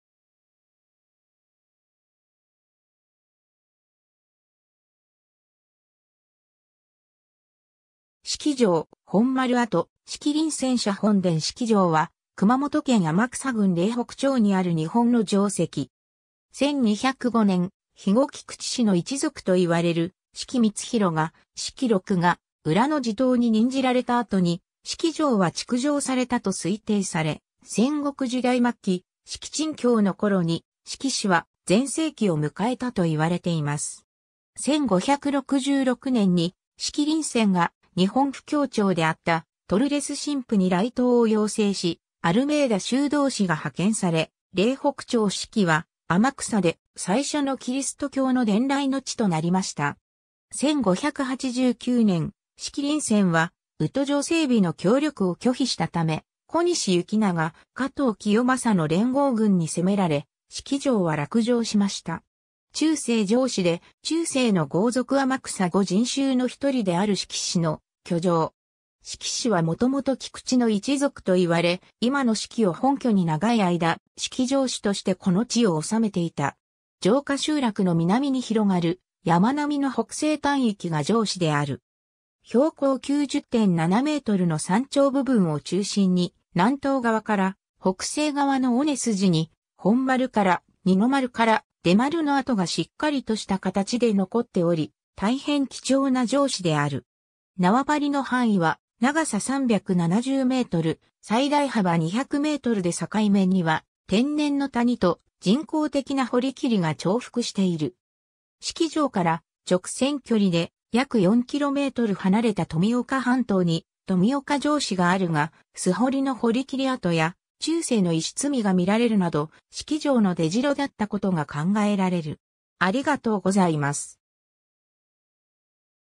志岐城
志岐城本丸跡 志岐麟泉社本殿 志岐城（しきじょう）は、熊本県天草郡苓北町にある日本の城跡。
 1205年、肥後菊池氏の一族といわれる志岐光弘が志岐6ヶ浦の地頭に任じられた後に志岐城は築城されたと推定され、戦国時代末期、志岐鎮経（麟泉）の頃に志岐氏は全盛期を迎えたといわれています。
1566年に、志岐麟泉が日本布教長であったトルレス神父に来島を要請し、アルメイダ修道士が派遣され、苓北町志岐は天草で最初のキリスト教の伝来の地となりました。
1589年、志岐麟泉は宇土城整備の協力を拒否したため、小西行長・加藤清正の連合軍に攻められ、志岐城は落城しました。
 中世城址で、中世の豪族天草五人衆の一人である志岐氏の居城。
志岐氏は元々菊池の一族と言われ、今の志岐を本拠に長い間、志岐城主としてこの地を治めていた。
城下集落の南に広がる山並みの北西端域が城址である。
標高90.7mの山頂部分を中心に南東側から北西側の尾根筋に「本丸」 - 「二の丸」 - 「出丸」の跡がしっかりとした形で残っており、大変貴重な城址である。
縄張りの範囲は、長さ370m、最大幅200mで境目には天然の谷と人工的な堀切が重複している。
志岐城から直線距離で、約4km離れた富岡半島に富岡城址があるが、素掘の堀切り跡や中世の石積みが見られるなど、志岐城の出城だったことが考えられる。